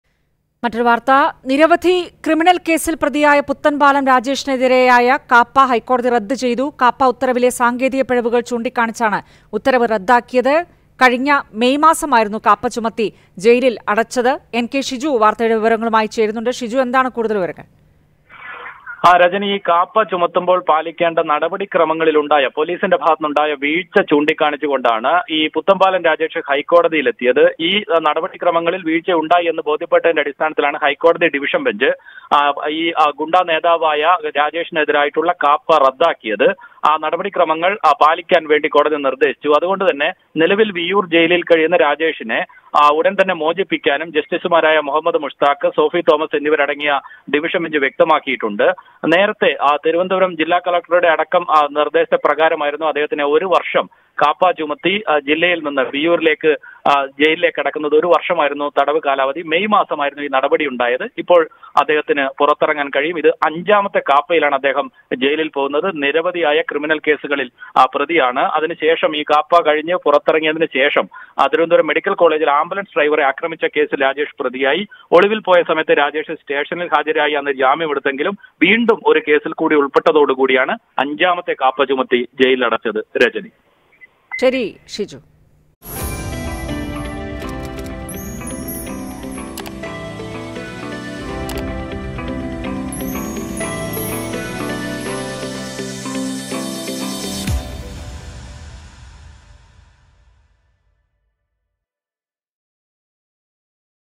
പുത്തൻപാലം രാജേഷിനെതിരായ കാപ്പ ഹൈക്കോടതി റദ്ദ് ചെയ്തു | Puthanpalam Rajesh
പുത്തൻപാലം രാജേഷിനെതിരായ കാപ്പ ഹൈക്കോടതി റദ്ദ് ചെയ്തു; കാപ്പ ഉത്തരവിലെ സാങ്കേതിക പിഴവുകൾ ചൂണ്ടിക്കാണിച്ചാണ് ഉത്തരവ് റദ്ദാക്കിയത്